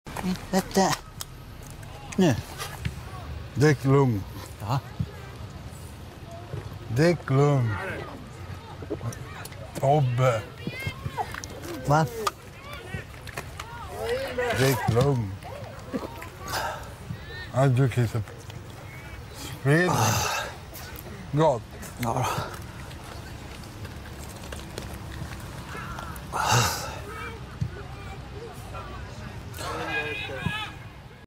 FREDAGSÖLEN | När man är lite sen till förfesten
När ölsuget kickar in!

Fredagsölen efter jobbet eller när man är lite sen till förfesten och vill komma ikapp.
Vem saknar inte Fredagsölen? Fredagsbiran lever vidare! När du är lite sen till förfesten och du får första ölen.